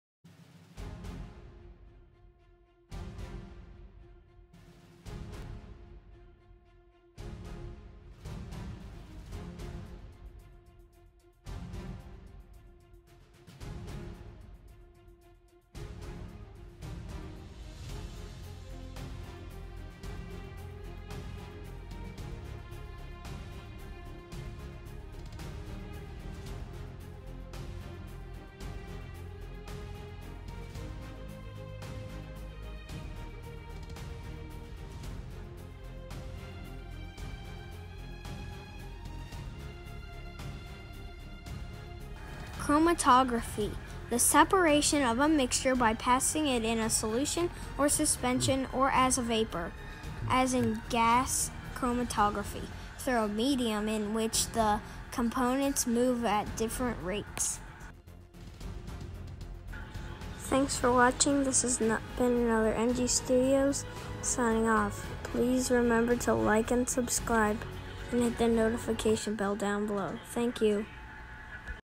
Chromatography!!!!
An easy, quick experiment to try chromatography